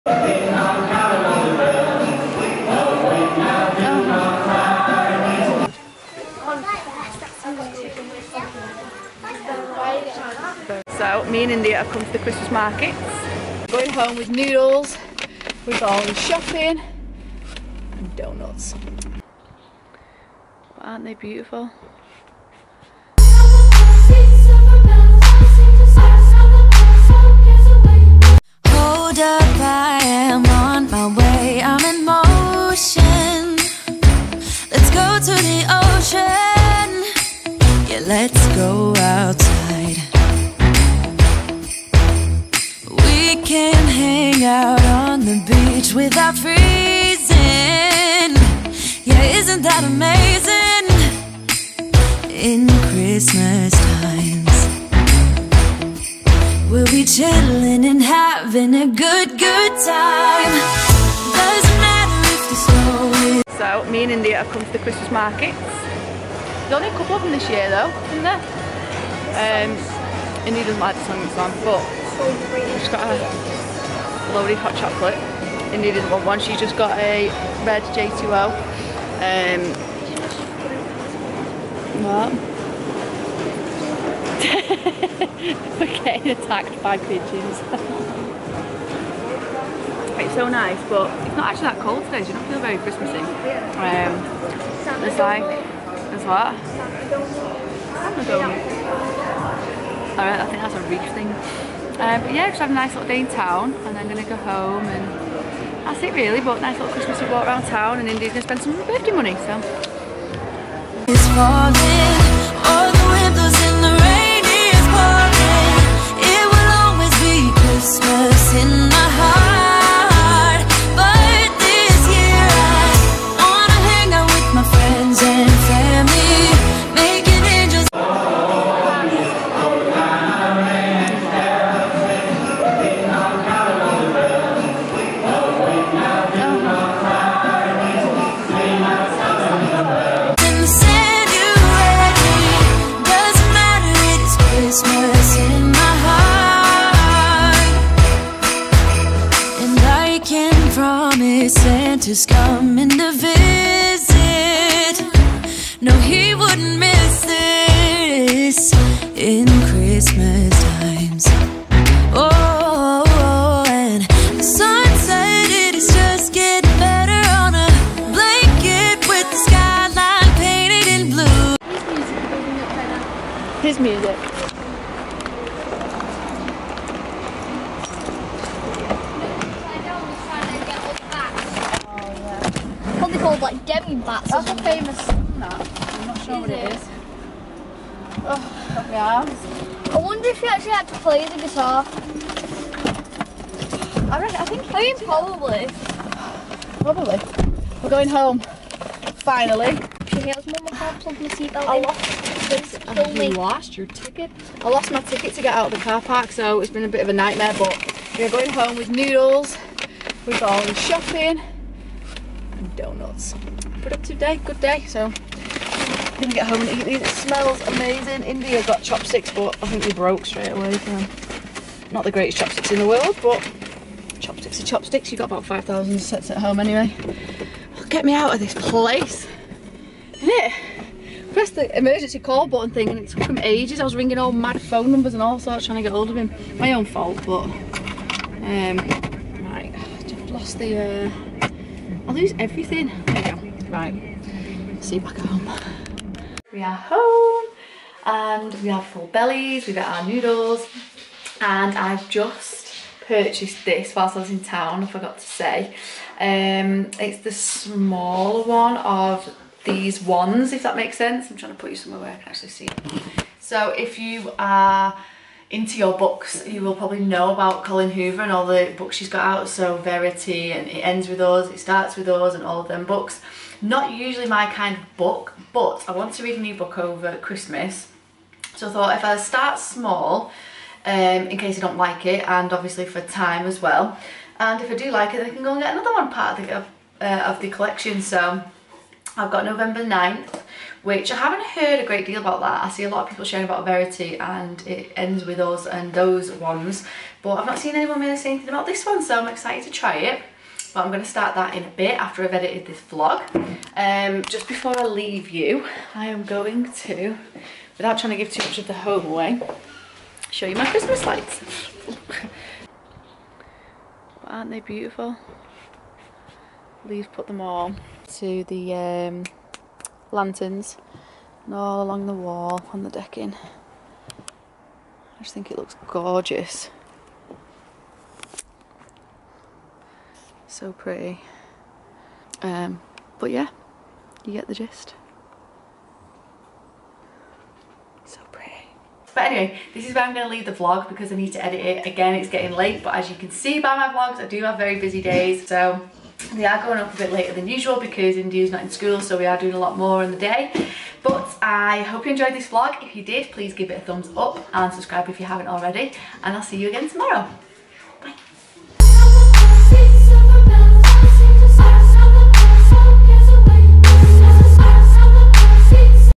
VLOG | CHRISTMAS MARKETS, FOOD, SHOPPING AND SANTA, VLOGMAS DAY 20
Me and India went to our town centre and spent some time in the Christmas markets. Then we went shopping, got lots of different foods and went home to our pretty Christmas lights :)

CHAPTERS
00:00 Intro
00:28 Christmas Markets
01:51 Shopping And Food Places
02:08 Santa
02:20 More Shopping And More Food Places
02:51 Back To The Car After I lost My Car Park Ticket
04:06 Back Home Collen Hoover Book Chat
05:06 My Outside Christmas Lights
05:39 Outro